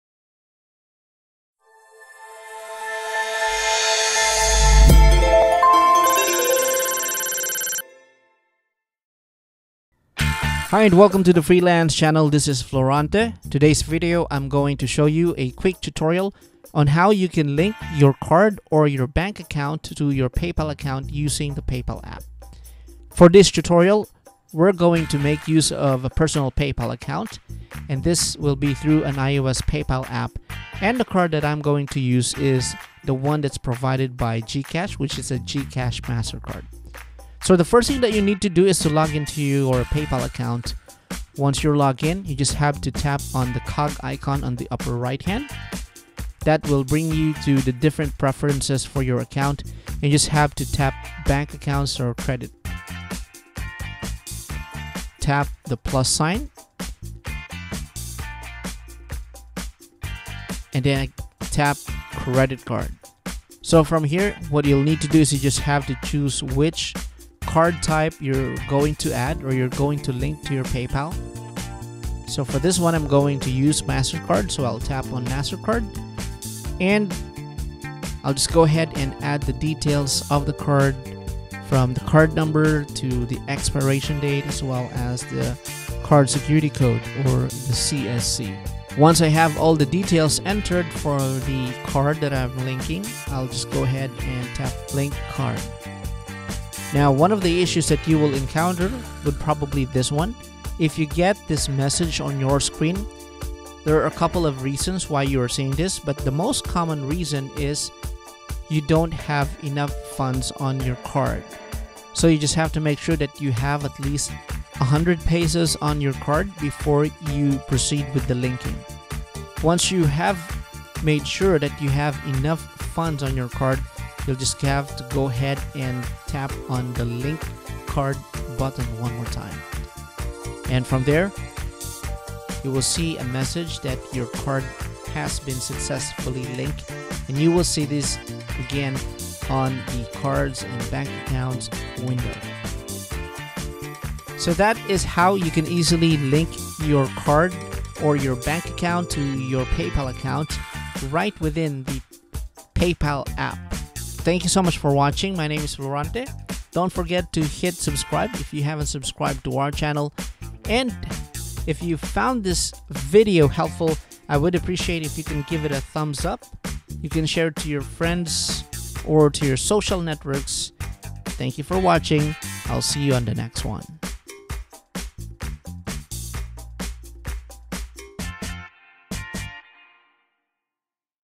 How to Link Debit or Credit Card to Paypal via Paypal App
Here's a quick video on how to link your Debit card Mastercard to your Paypal account using the mobile app from Paypal.

If you are into podcasting, check out my services and my podcast related channel